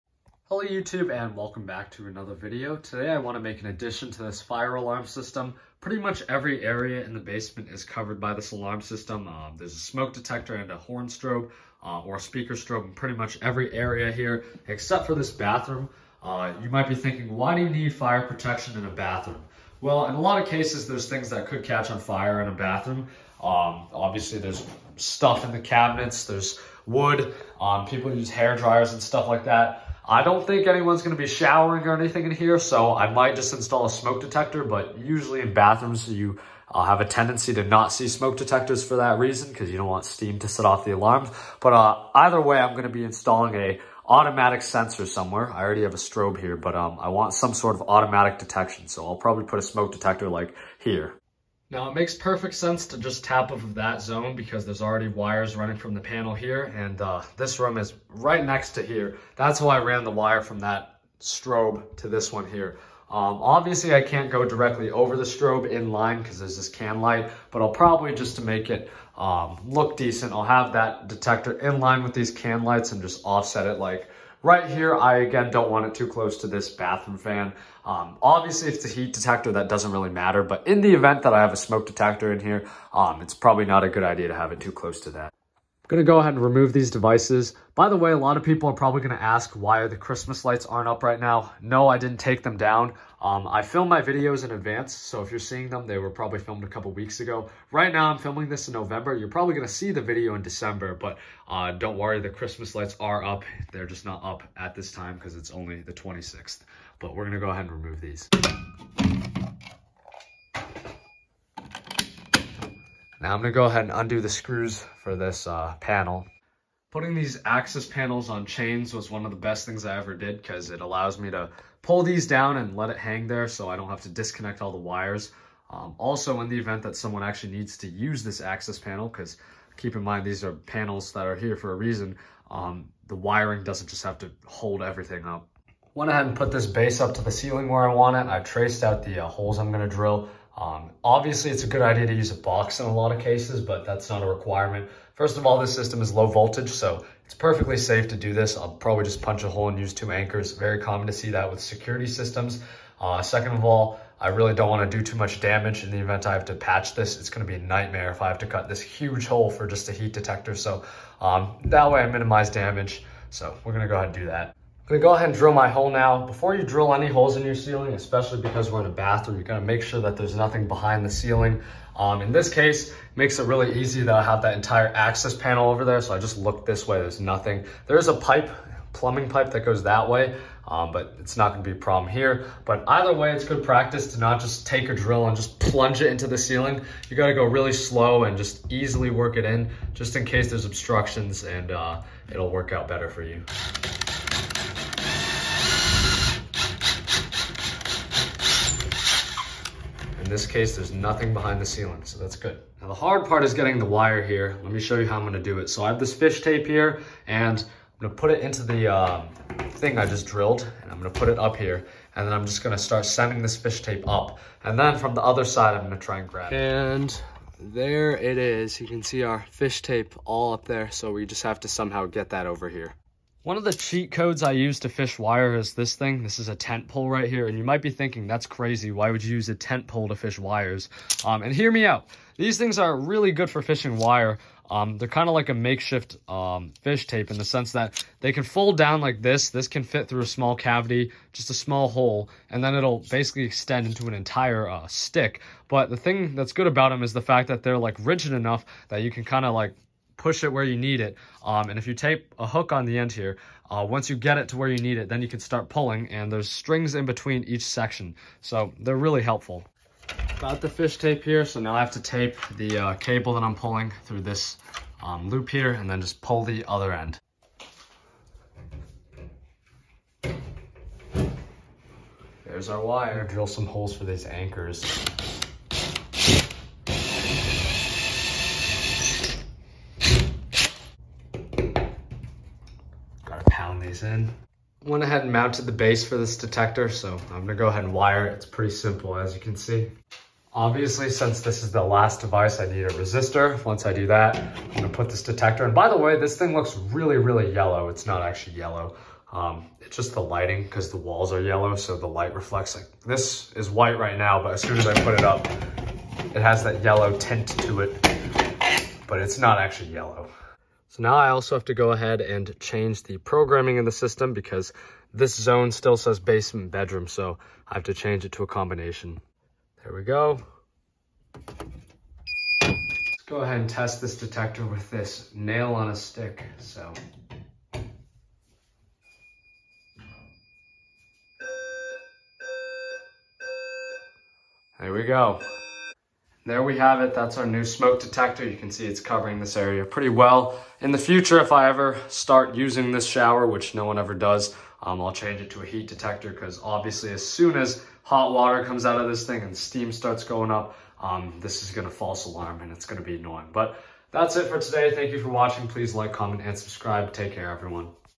Installing a Smoke Detector on the Basement Fire Alarm System
Process of adding automatic detector coverage in the basement bathroom. Usually, you would use a heat detector, but in this case, the shower is never used and it’s more or less a storage room, so a smoke detector was suitable.